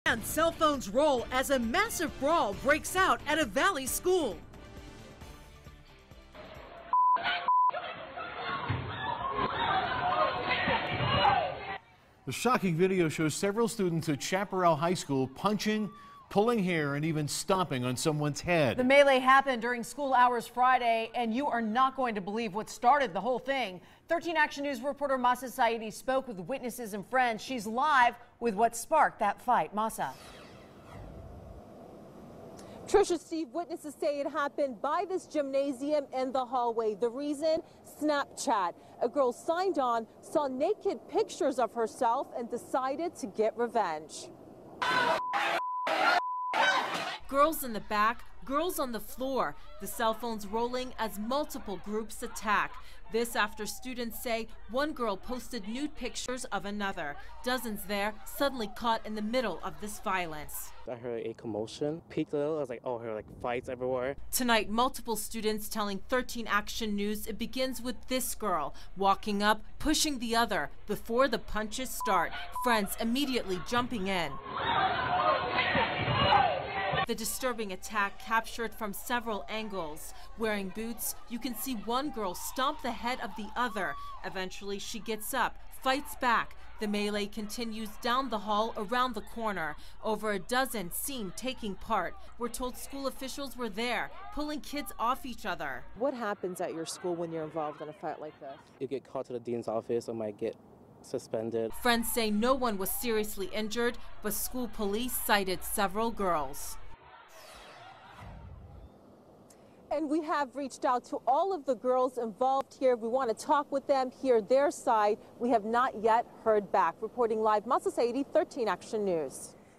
Disturbing brawl at high school caught on camera
Friends tell 13 Action News the fight was actually sparked on Snapchat.  Mahsa Saeidi reports.